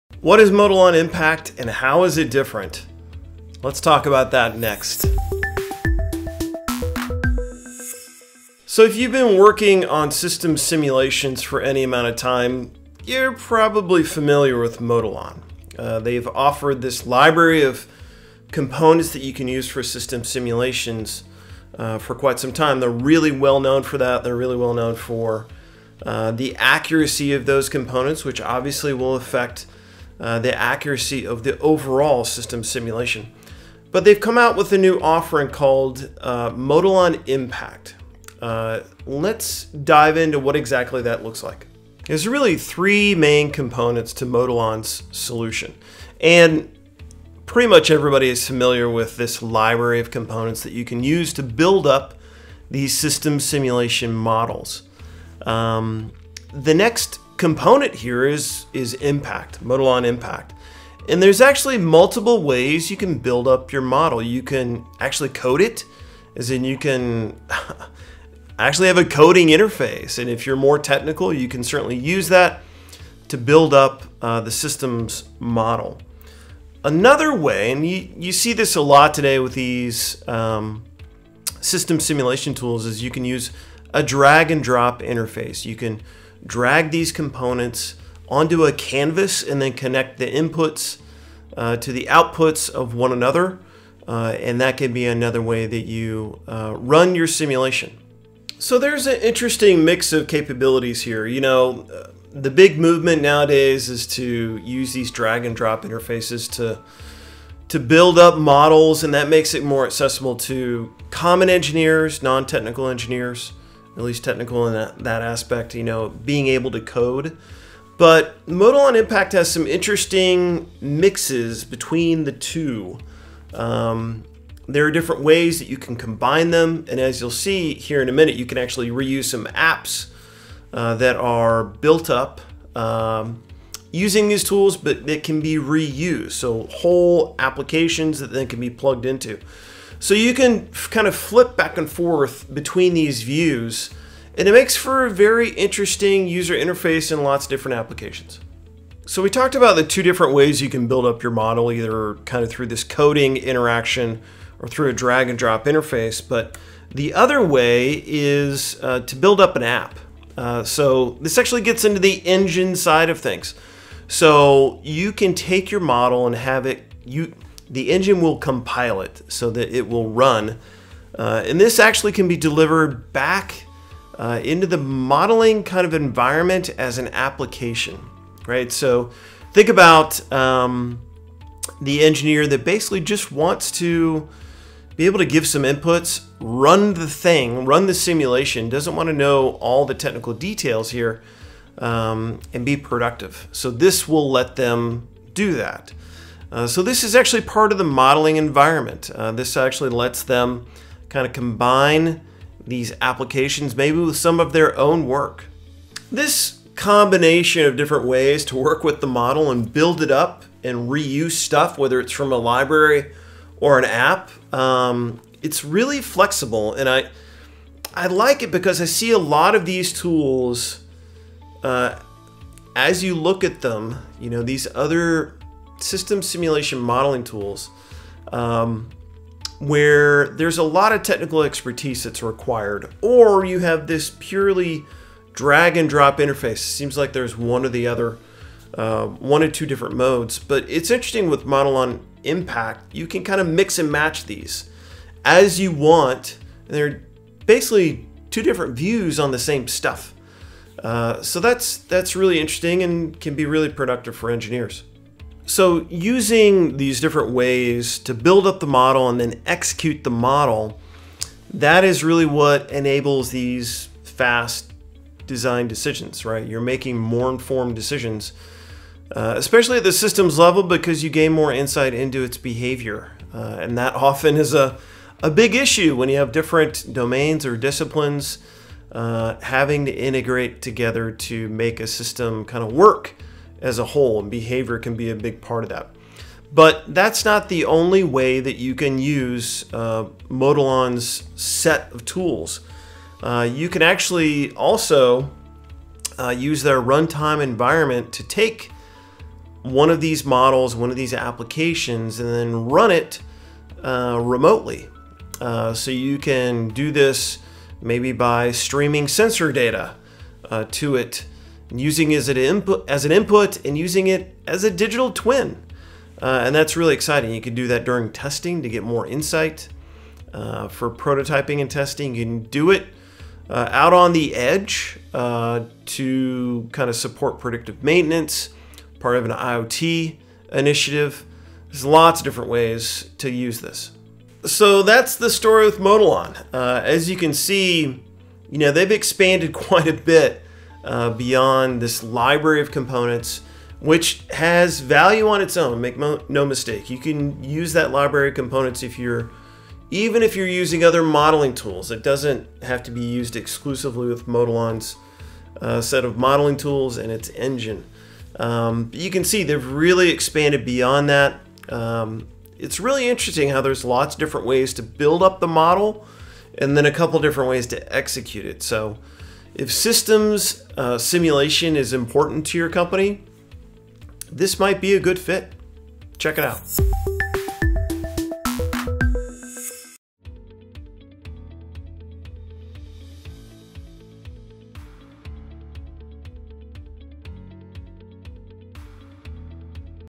What is Modelon Impact?
Modelon has offered a library of components that you can use for system simulations for quite some time. They're really well known for the accuracy of those components, which obviously will affect the accuracy of the overall system simulation. But they've come out with a new offering called Modelon Impact. Let's dive into what exactly that looks like.